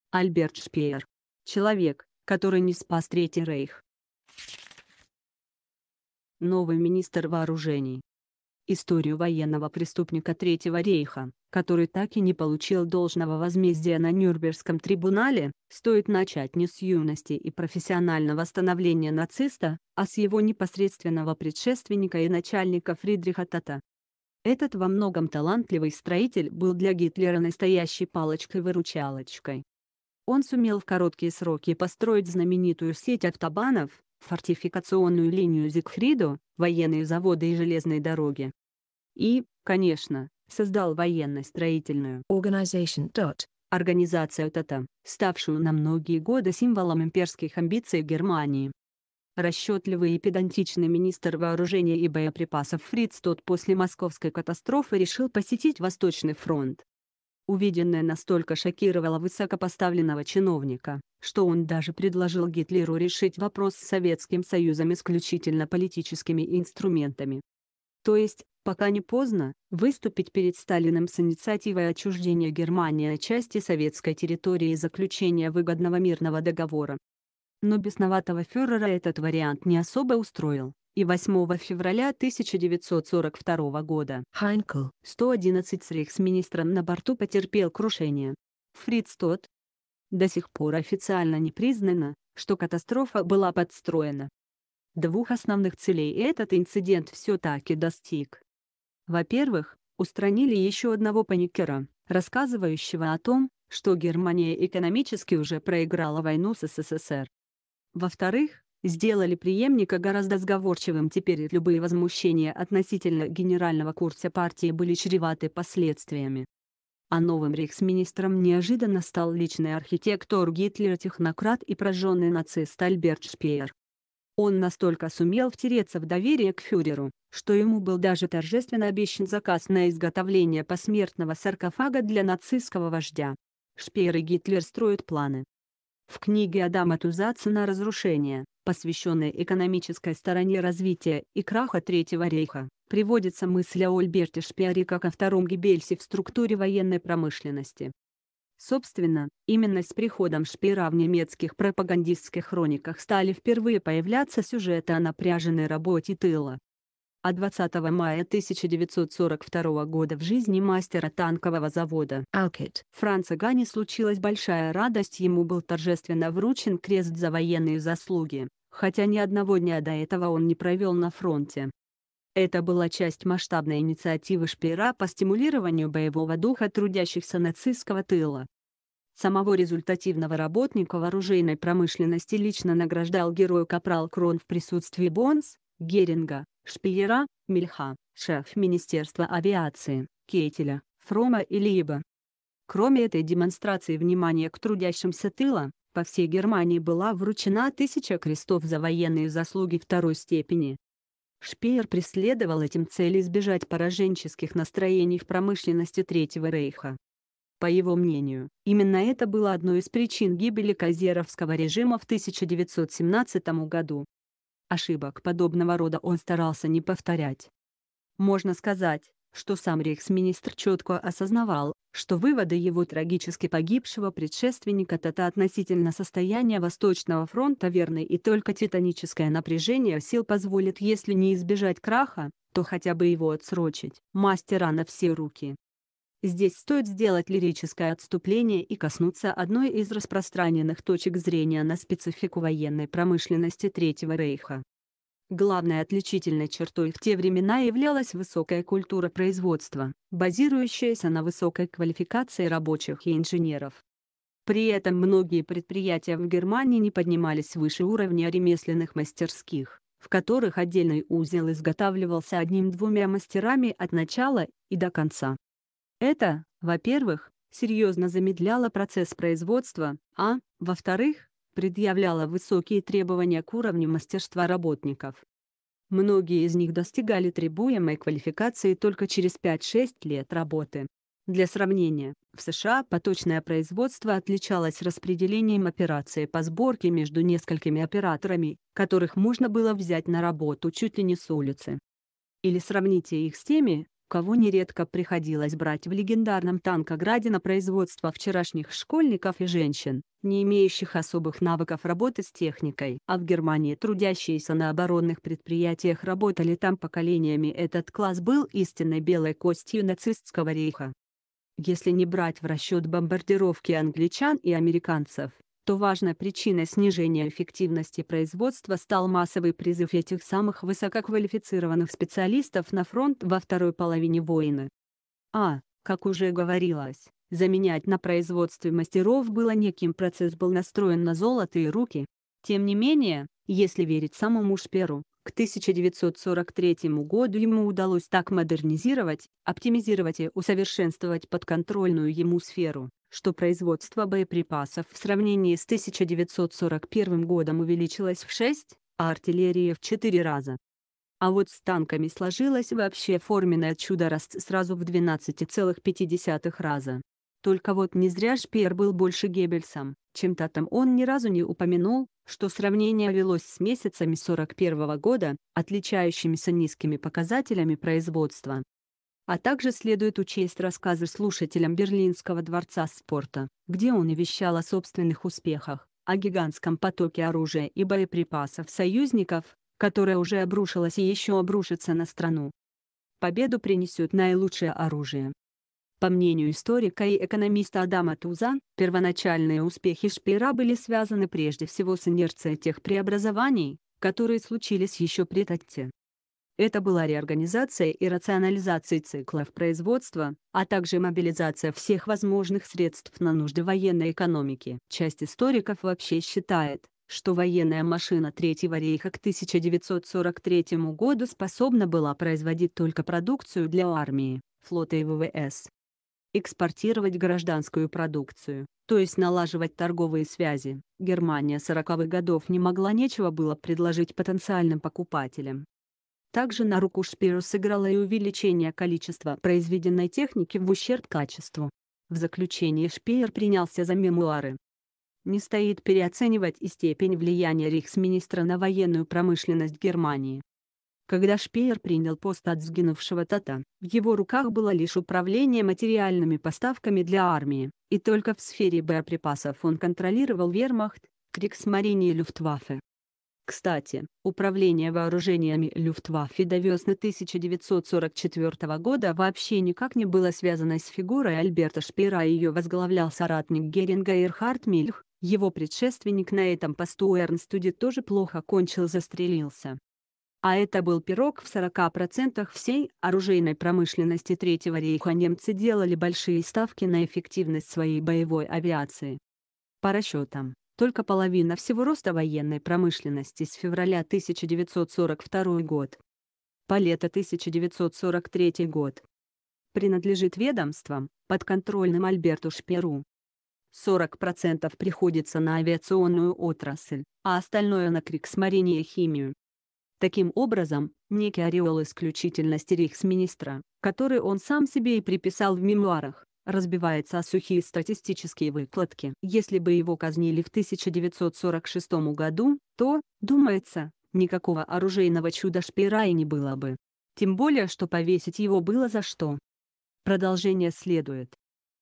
Альберт Шпеер. Человек, который не спас Третий рейх
Альберт Шпеер . Человек, который не спас Третий рейх Новый министр вооружений Историю военного преступника Третьего рейха, который так и не получил должного возмездия на Нюрнбергском трибунале, стоит начать не с юности и профессионального становления нациста, а с его непосредственного предшественника и начальника Фридриха Тодта . Этот во многом талантливый строитель был для Гитлера настоящей палочкой-выручалочкой . Он сумел в короткие сроки построить знаменитую сеть автобанов, фортификационную линию З